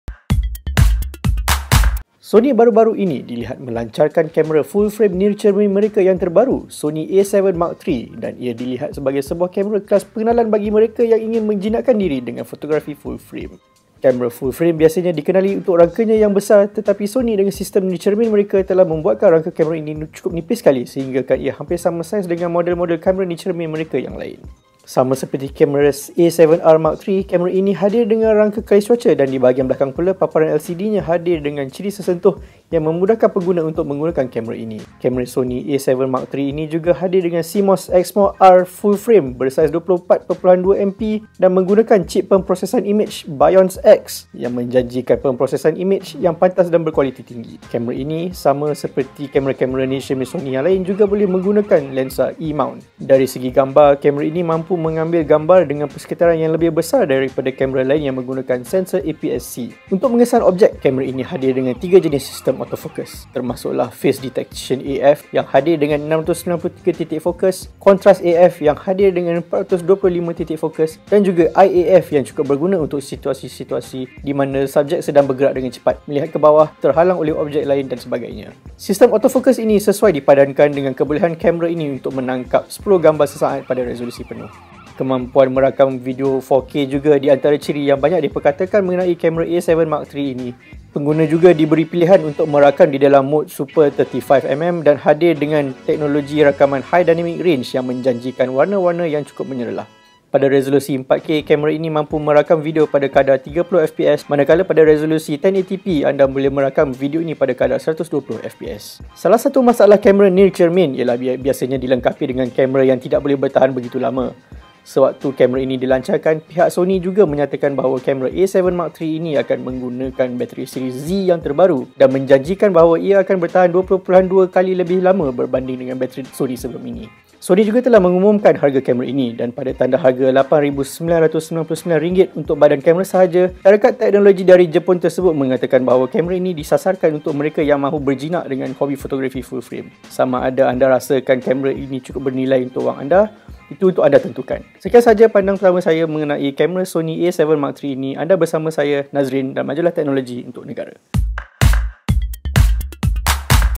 Pandang Pertama : Sony A7 III
Sony Malaysia memperkenalkan kamera Full Frame terbaru mereka, Sony A7 III untuk pasaran tempatan baru-baru ini. Ia hadir dengan sensor Full Frame 35mm 24.2MP bersama-sama dengan cip pemprosesan imej BIONZ-X mereka yang terbaru, yang juga mampu mengambil video pada resolusi 4K. Pada harga RM8999 untuk badan kamera sahaja, Sony juga menyatakan bahawa kamera ini ialah kamera Full Frame pertama mereka yang dibina khusus untuk mereka yang ingin berjinak-jinak dengan fotografi Full Frame.